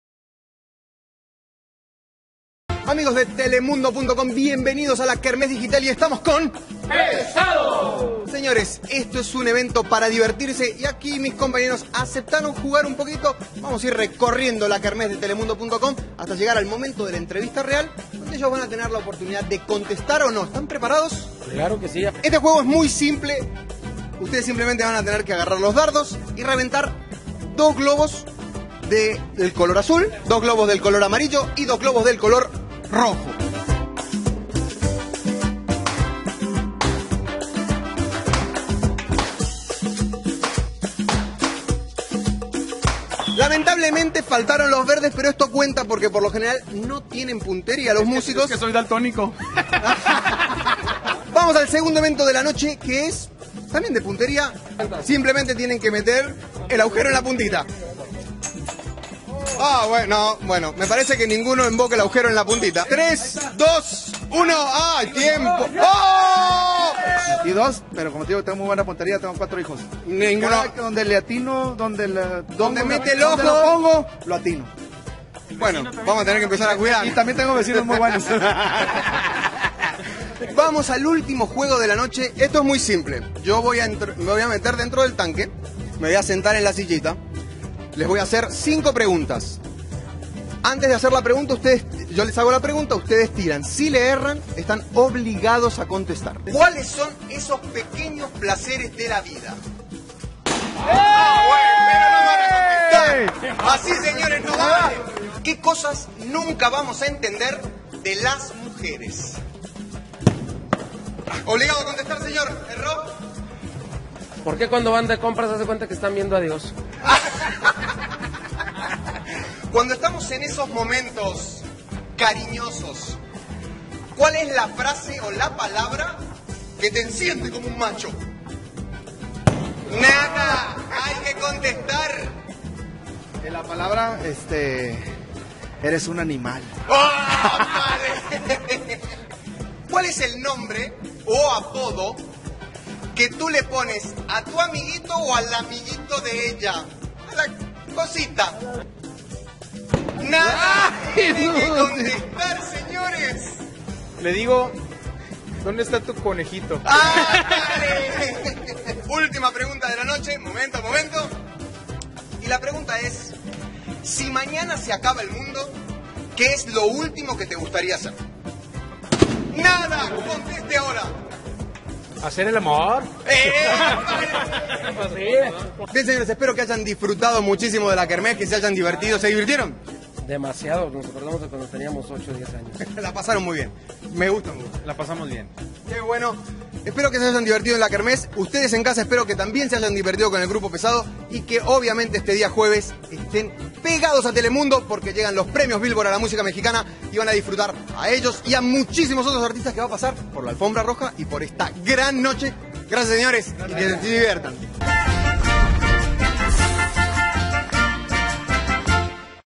ROBERTO BECHO PEREZ Grupo Pesado En Telemundo En La Kermesse Billboard Los Angeles
Una Entrevista Mas Del Grupo Pesado Previo A Los Premios Billboard En Los Angeles california 16 Oct 2012 De Kermesse En telemundo Con MaxxGuetta Suerte Pesado En Los Premios